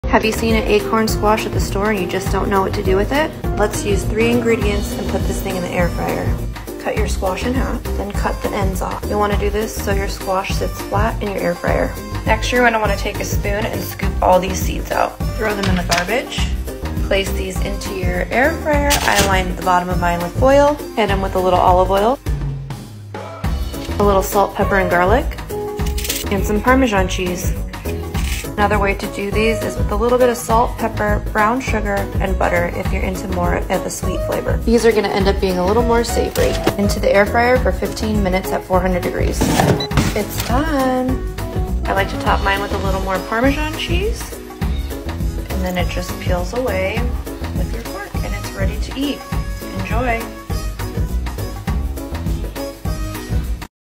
Air fried acorn squash
Acorn squash cooked in the air fryer:

-1 Acorn Squash cut in half
-EVOO
-Salt, pepper, garlic all to taste (just a few shakes)
-Parmesan cheese
Clean and cut your acorn squash in half. Clean the seeds out and score your squash. Drizzle with EVOO (I’m using a garlic flavor but it can be unflavored). Sprinkle on salt, pepper and garlic powder. Put into the air fryer at 400° for 15 minutes. Add more Parmesan cheese if you’d like. Enjoy!